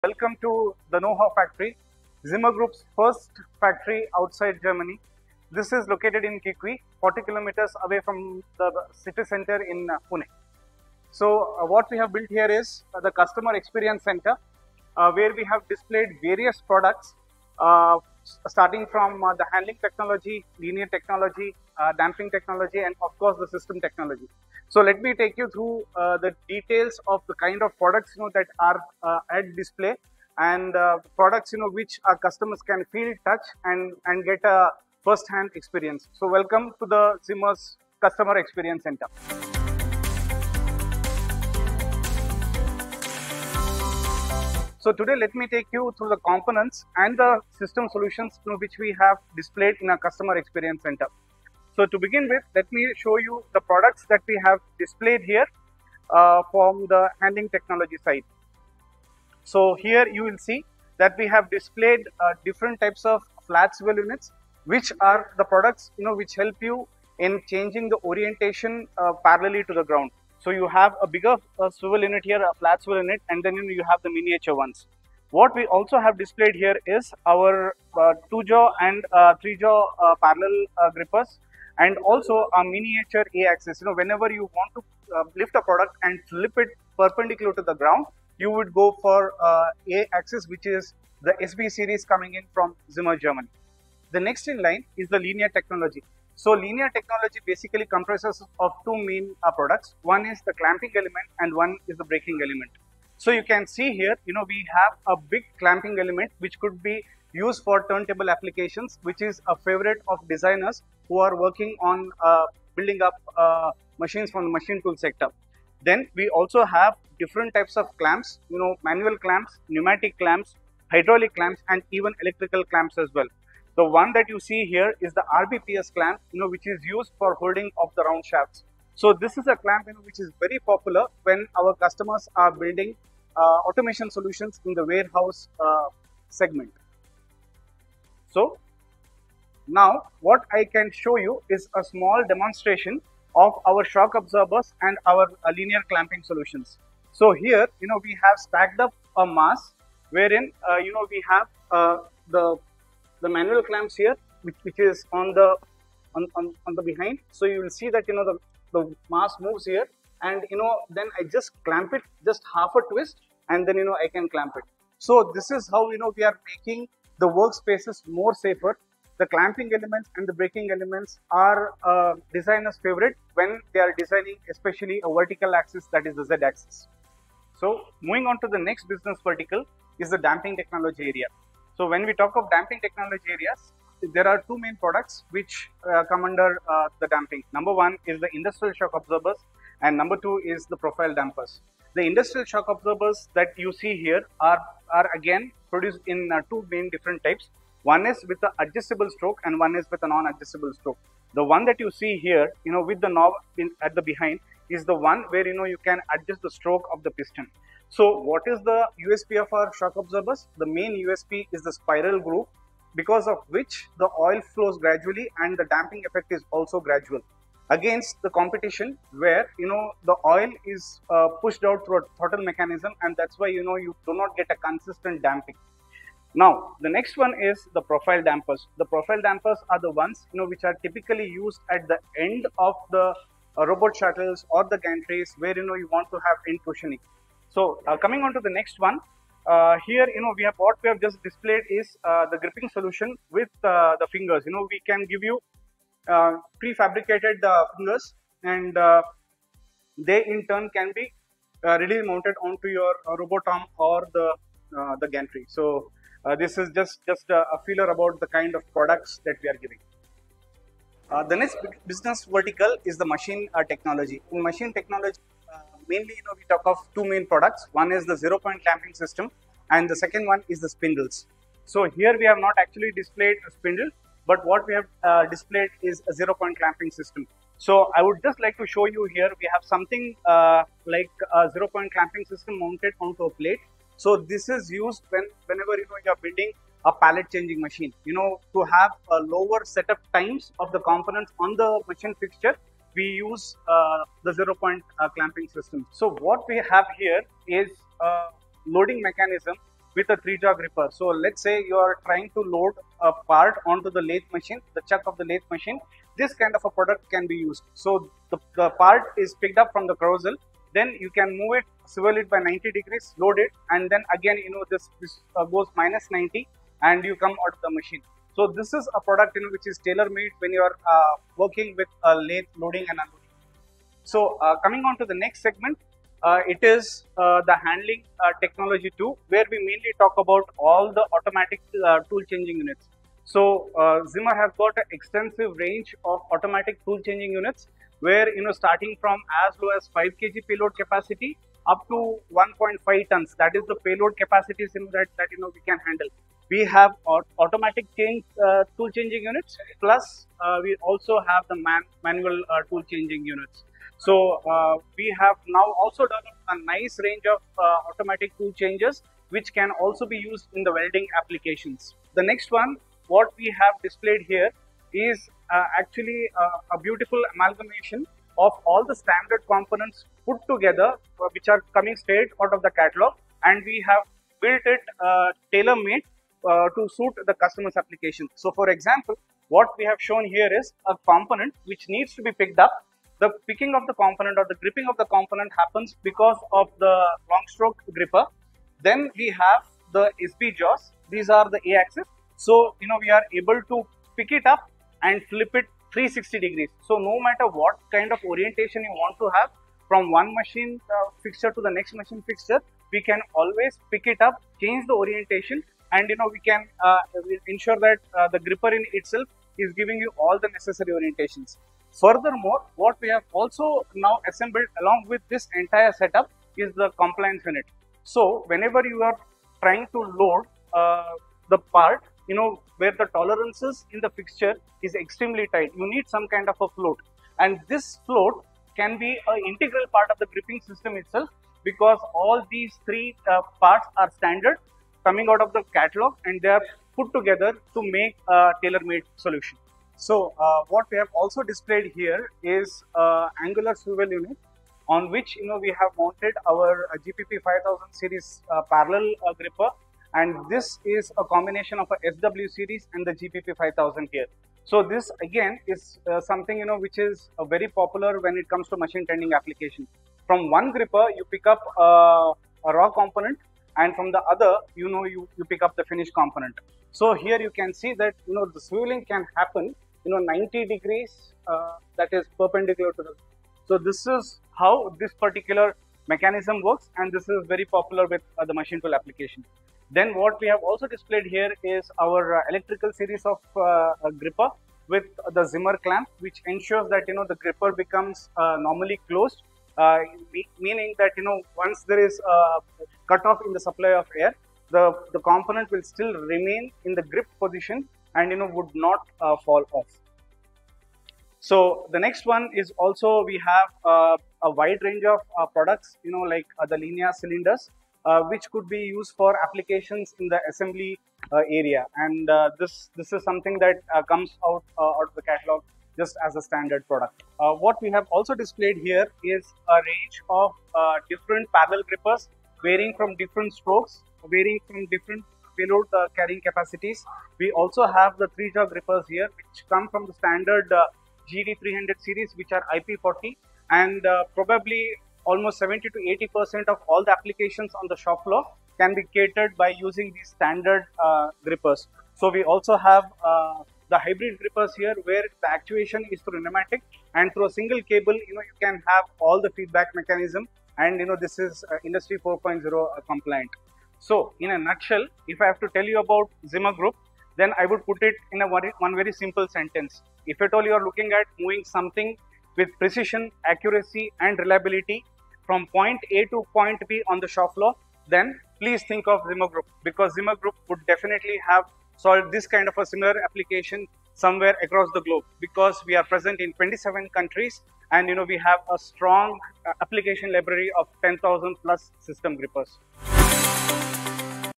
The future of manufacturing with Zimmer Group India's new factory
Step into the future of manufacturing with Zimmer Group India's new factory in Kikvi, Pune!

Hrishikesh Kulkarni, COO of Zimmer Automation Pvt Ltd, takes you on a journey through our cutting-edge products and solutions. Our Customer Experience Center showcases a diverse array of products - from handling to damping and system technologies. Have a look yourself!

With our newly inaugurated factory, we reaffirm our dedication to providing advanced solutions for manufacturing, assembly, and automation needs. Our global presence, coupled with an extensive application library, ensures precision, reliability, and accuracy in every solution we offer.

Join us as we revolutionize the manufacturing landscape, enhancing operational efficiency for customers worldwide.

Thanks to Magic Wand Media for the video!